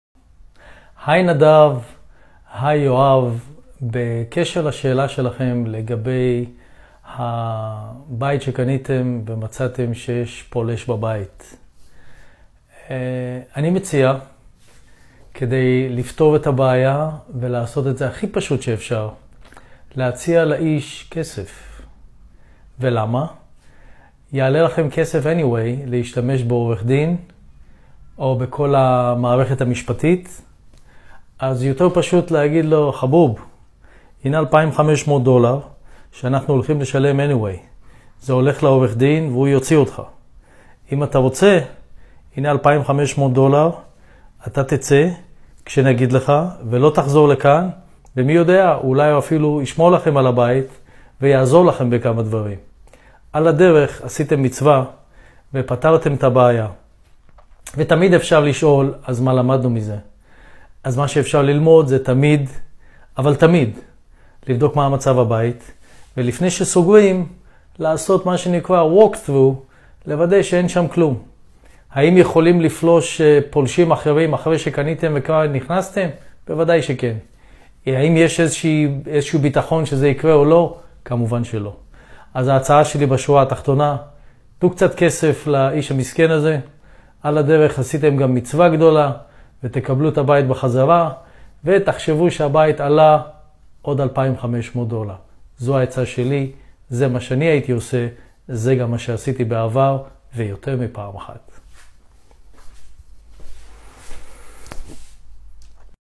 David Lapid - דויד לפיד - איך לטפל בפולש לנכס - פורום נדל"ן ארצות הברית בפייסבוק
פורום נדל"ן ארצות הברית בפייסבוק